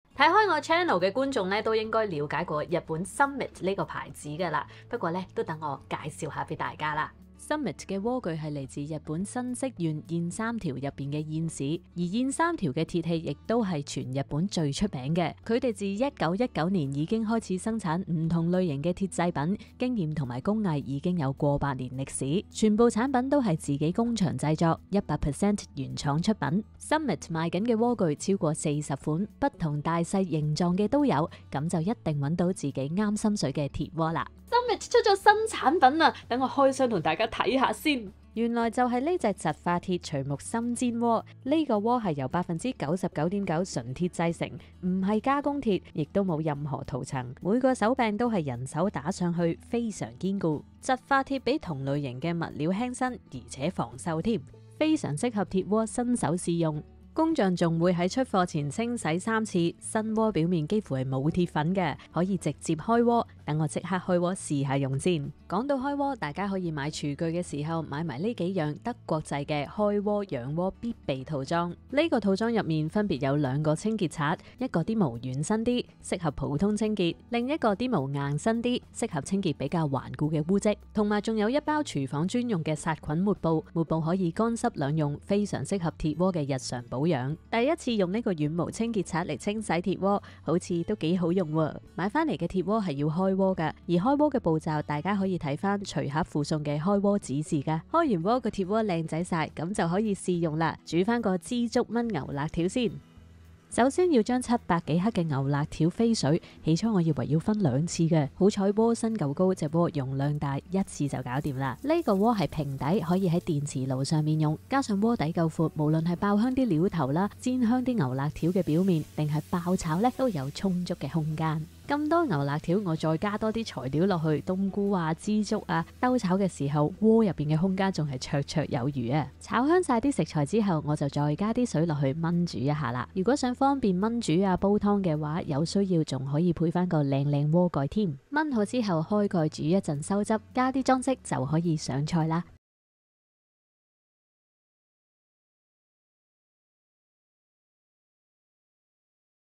Summit x McFatty 麥花田【🥘一鍋搞定！柱候枝竹牛肋條的簡易做法】窒化槌目深煎鍋 烚,炒,燜煮 一次過完成
想要在家輕鬆做出美味的柱候枝竹牛肋條嗎？
今天麥花田@McFattyHK 將教你如何利用窒化槌目深煎鍋，一鍋到底製作出這道美味料理！🌟

窒化槌目深煎鍋不僅擁有超大的容量，還能讓你在烹調過程中無需轉換鍋具。從爆香到燜煮，所有步驟都能在同一個鍋裡完成，簡單又方便！👍

深煎鍋高達8.5CM, 可以一次性放入大量食材, 700克牛肋條一次過汞水都無問題
爆香料頭再直接煎香牛肋條, 最後加入枝竹位置都綽綽有餘!

來自日本新潟縣燕市的專業鐵鍋Summit Japan。
🏡 家‧傳承溫暖